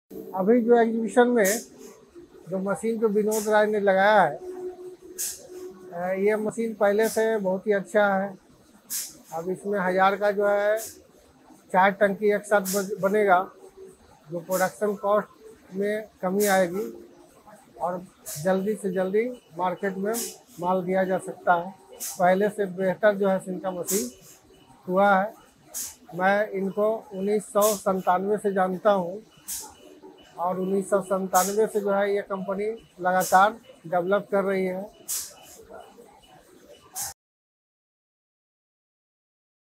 Freedom Plast Bihar sharing their experience with team Vinodrai & our Rotational moulding machines.
Freedom Plast Bihar sharing their experience with team Vinodrai and our Rotational moulding machines.